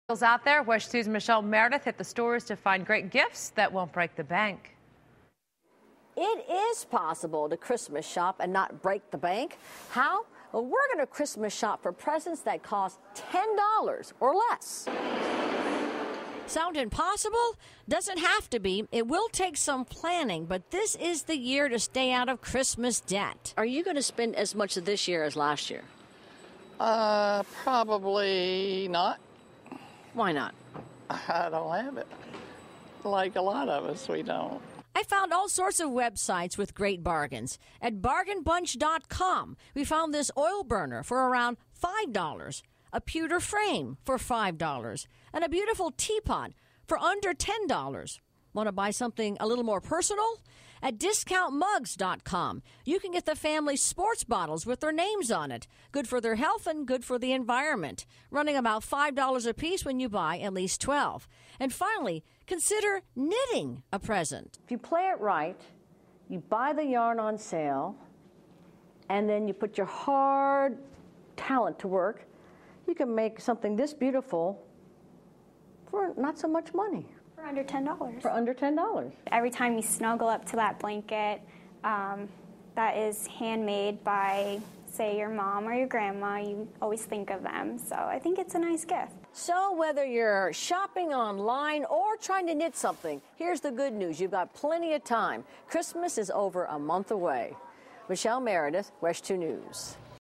Shop For Gifts Under $10
WESH 2 News reporter Michelle Meredith has tips on finding holiday gifts for less than $10.